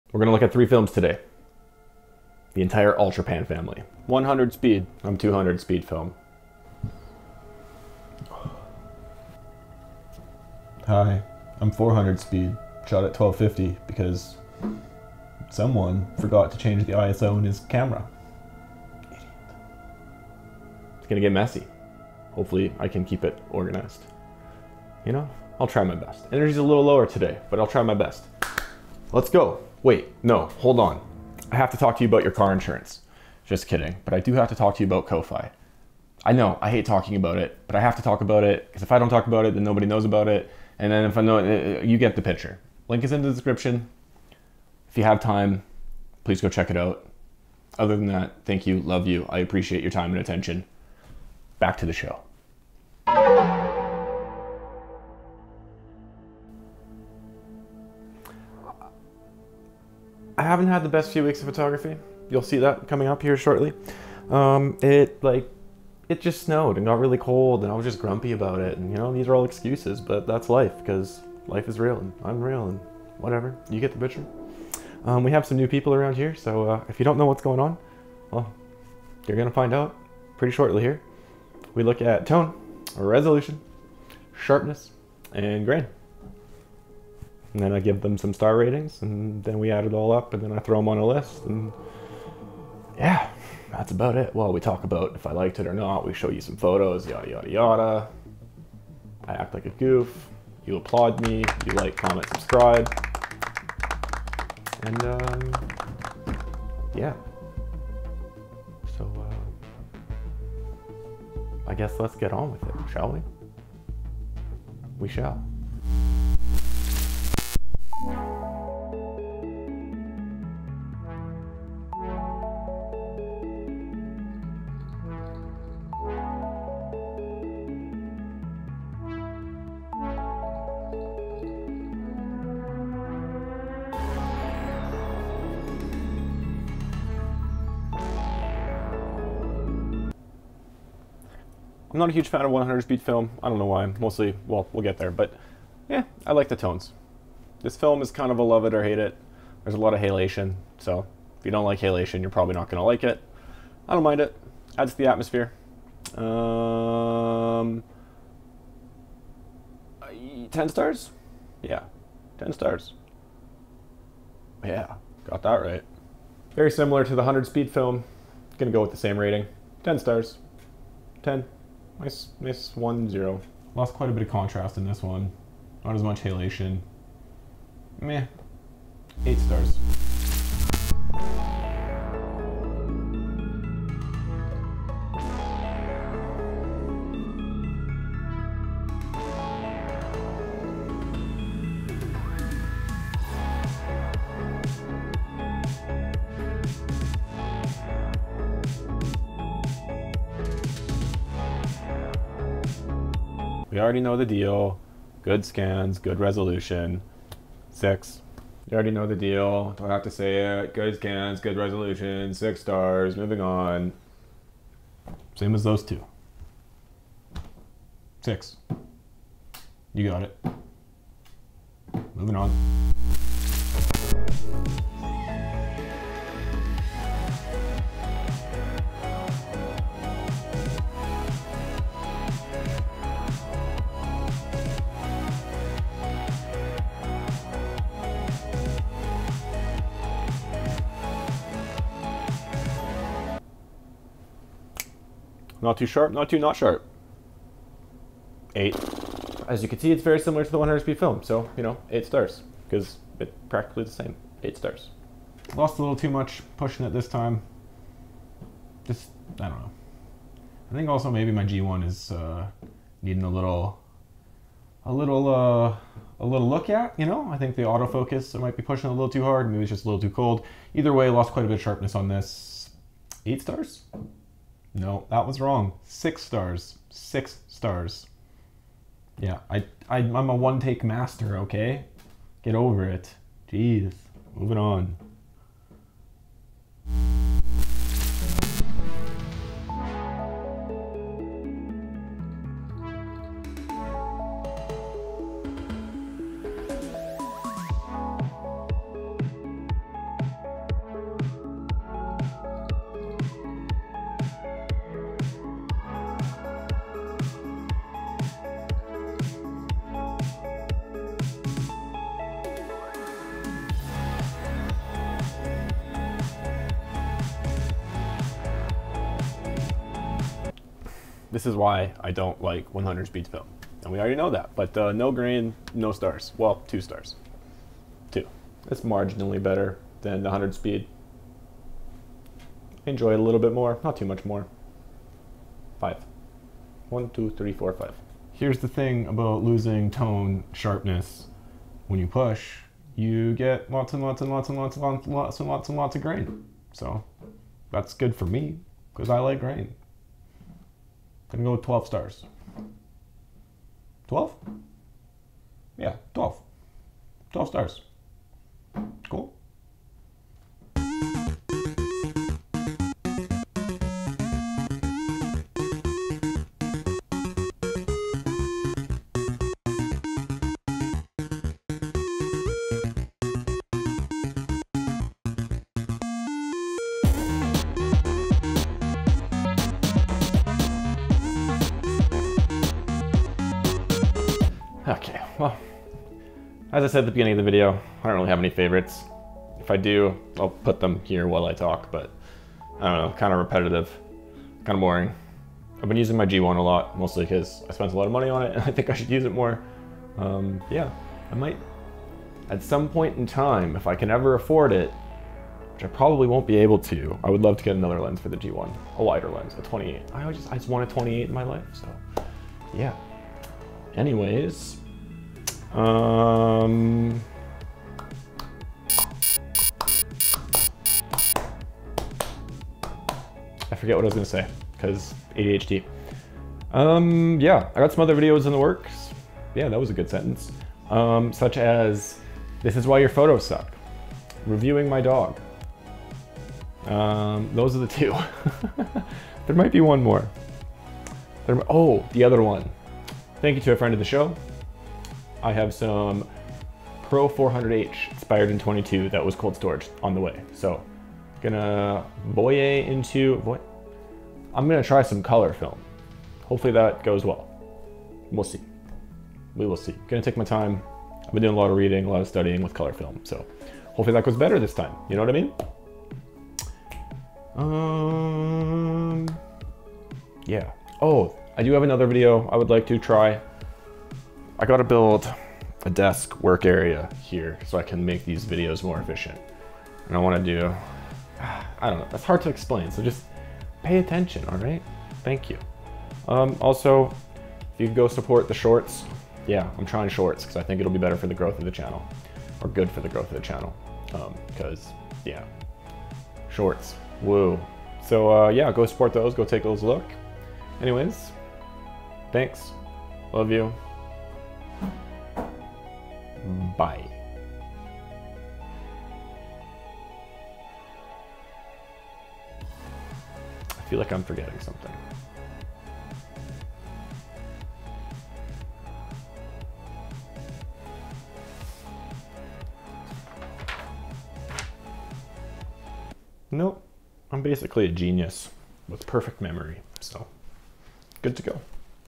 whats up with the ultrapan family?
Today we look at three films. Yes thats right 3 of them. Look Ultrapan is cheap and it's what I had so here we are. Not to worry! We have some fun stocks going soon...

Film Stocks - Flic Film Ultrapan 100, 200 and 400 @ 1250
Camera - Contax G1
Developer - Dills homemade Kodak D76
Scan - Fujifilm GFX100s w/ Schneider macro 90 4.5 enlarger lens on bellows
Conversion - Negative Lab Pro